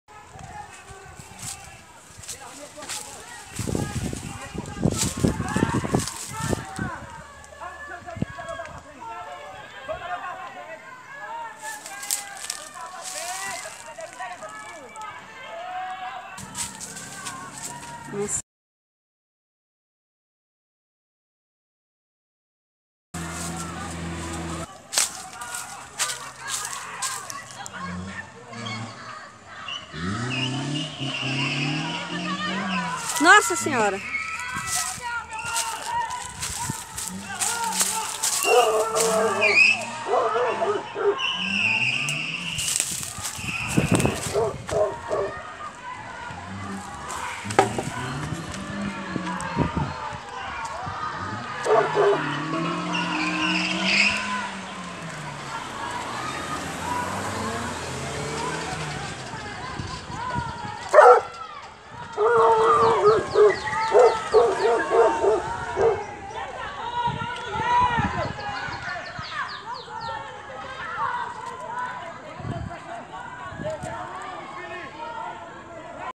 BORRAZÓPOLIS - Briga generalizada próximo ao Lago Municipal - Veja vídeo
Moradores registraram uma grande confusão que aconteceu na quadra de esportes abandonada, que fica atrás do Clube da Cultura, ao lado do Lago Municipal de Borrazópolis. Era por volta das 15:30 horas, quando começou a confusão. Há relatos que as pessoa que ocupavam o local, seriam ciganos ou índios, mas não conseguimos confirmar. Em meio a confusão, uma pessoa saiu com um carro branco e chegou a atropelar outras, mas sem gravidade. Um veículo foi amassado e houve muita gritaria. Logo após o carro branco deixar a quadra, o motorista de um Gol, saiu com um facão na mão dizendo que iria acertar as contas. A Polícia Militar foi acionada, mas ainda não gerou o boletim de ocorrências. Nossa reportagem foi ao local e fomos abordado por uma mulher. Ela pedia que chamássemos a Polícia novamente, porque uma das envolvidas havia furado o seu olho. Depois de muito bate boca e troca de agressões, todos deixaram a quadra. Veja o vídeo.